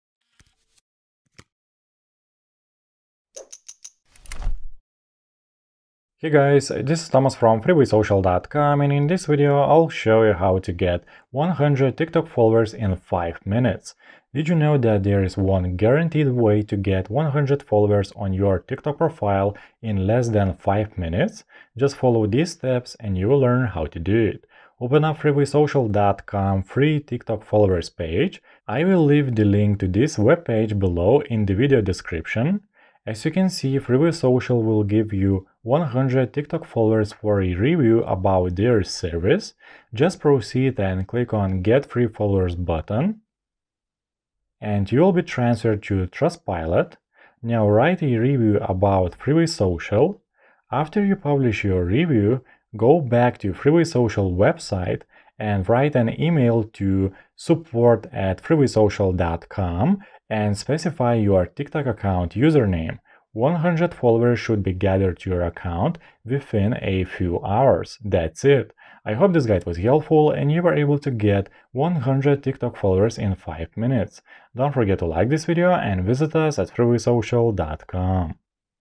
How To Get 100 TikTok Followers In 5 Minutes
In this video I am going to show you how to get 100 TikTok followers in 5 minutes.

Did you know that there is one guaranteed way to get 100 followers on your TikTok profile in less than 5 minutes?

Just follow these steps and you will learn how to do it:

2. As you can see FreewaySocial will give you 100 TikTok followers for a review about their service.
3. Just proceed and click on “Get free followers” button and you will be transferred to “TrustPilot”.
4. Now write a review about FreewaySocial.
5. After you publish your review, go back to FreewaySocial website and write an e-mail to and specify your TikTok account username. 100 followers should be gathered to your account within a few hours.

That’s it!

I hope this guide was helpful and you were able to get 100 TikTok followers in 5 minutes. Don’t forget to like this video and visit us